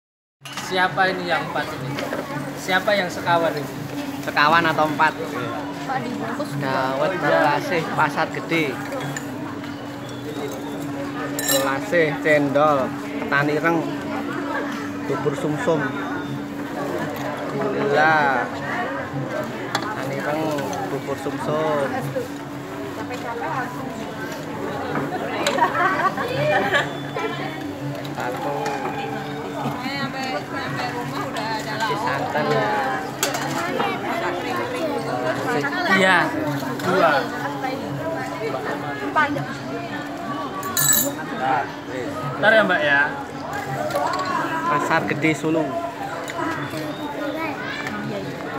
Dawet Telasih Pasar Gede Solo
Pasar Gede Solo jajan Dawet Telasih.
17 Februari 2018. Sendirian... tenangkan pikiran setelah menikmati lampion imlek di sekitar pasar gede solo.